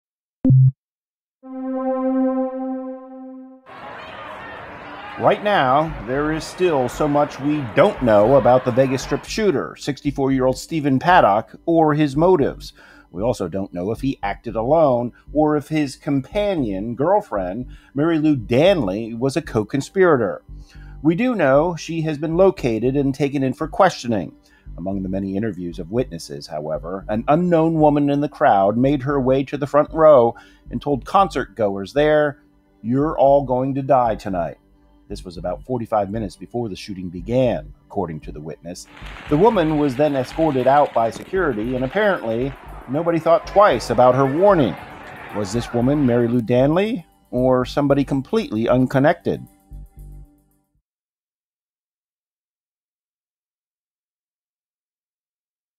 Las Vegas shooting: Witnesses say woman warned 'you're all going to die tonight'
At least 50 people died and hundreds more were wounded when gunman Stephen Paddock opened fire during Jason Aldean's performance at the Route 91 Harvest Music Festival in Las Vegas.